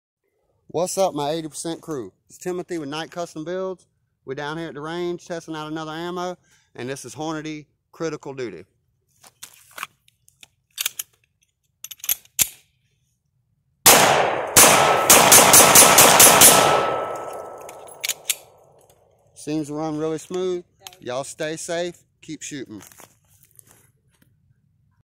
Hornady Critical Duty!! 🔥 Self Defense Ammo out a Polymer80!
Hornady Critical Duty seems to be a great round for self defense! Are y'all running this round?